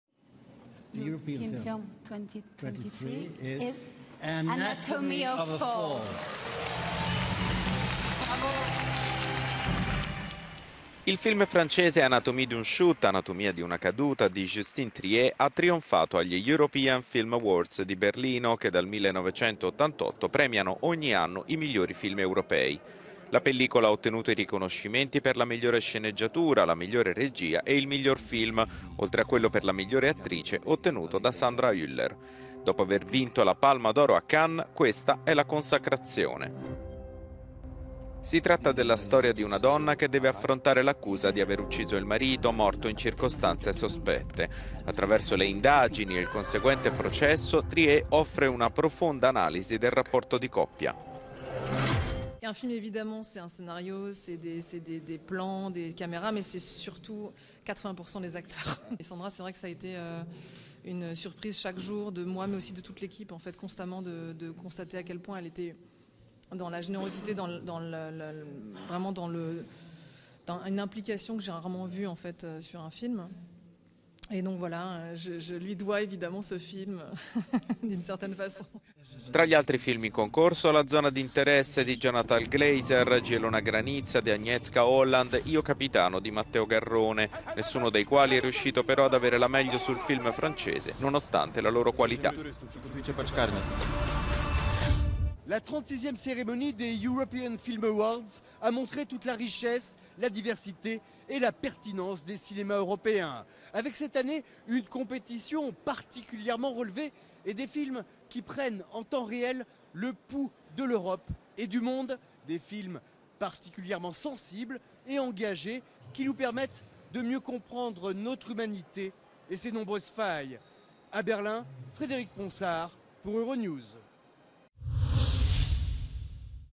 European Film Awards, il trionfo di "Anatomia di una caduta"
Agli European Film Awards, "Anatomia di una caduta" fa incetta di premi, nonostante la qualità delle altre pellicole in concorso